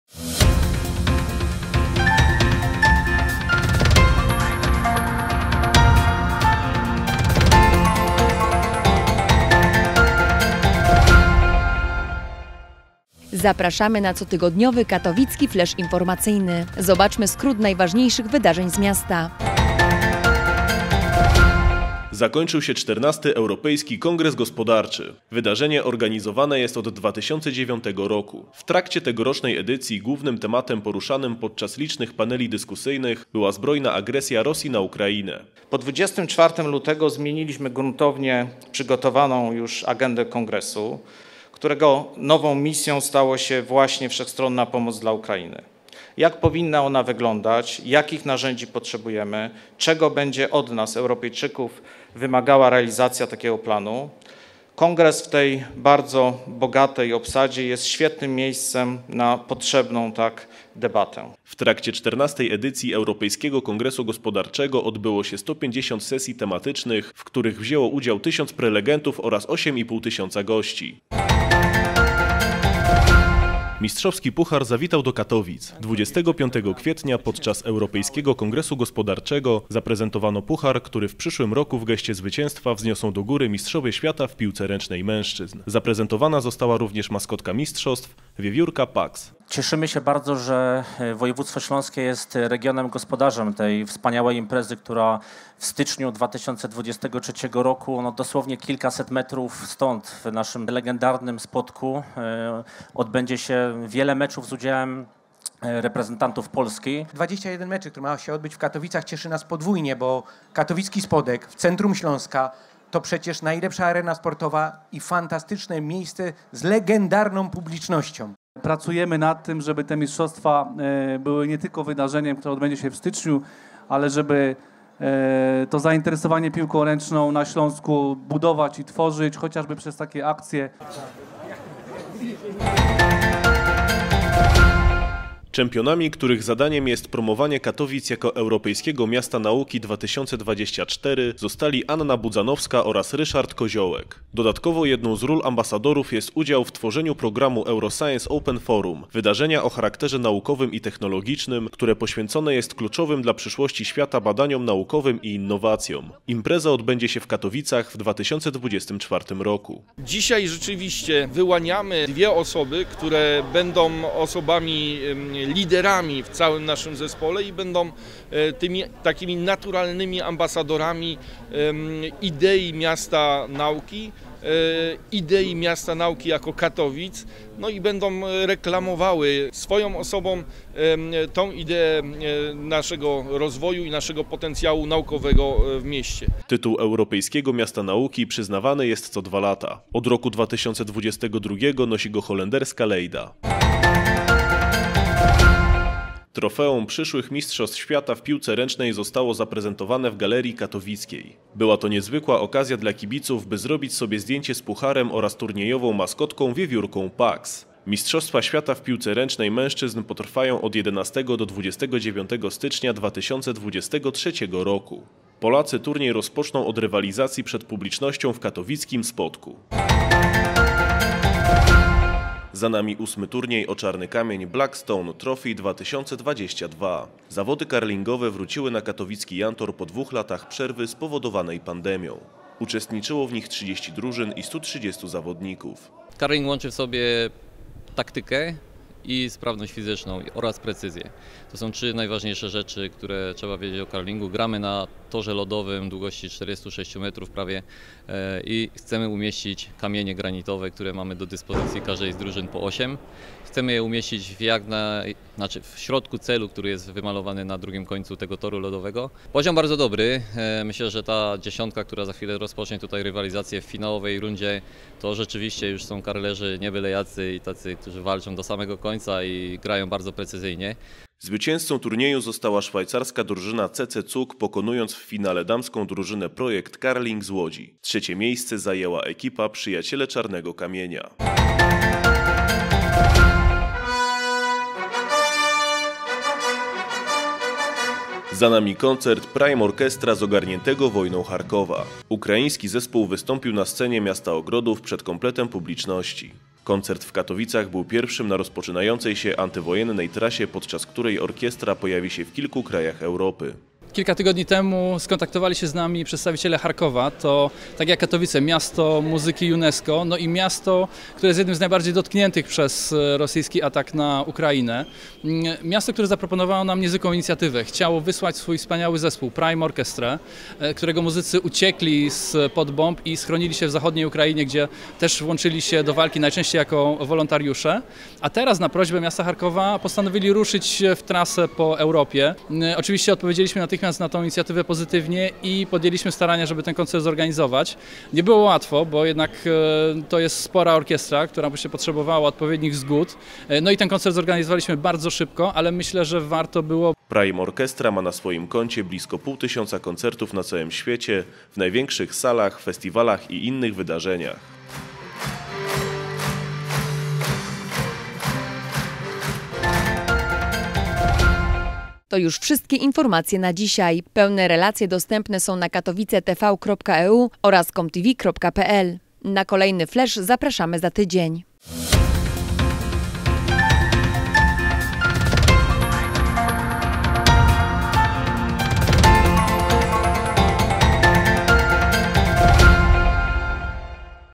KATOWICKI FLESZ INFORMACYJNY 3.05.2022
W tym wydaniu:
- XIV Europejski Kongres Gospodarczy
- Zaprezentowano Puchar Mistrzostw Świata w Piłce Ręcznej
- Championi Europejskiego Miasta Nauki Katowice 2024
- Puchar MŚ w Galerii Katowickiej
- Turniej Black Stone Trophy 2022
- Antywojenny koncert Prime Orchestra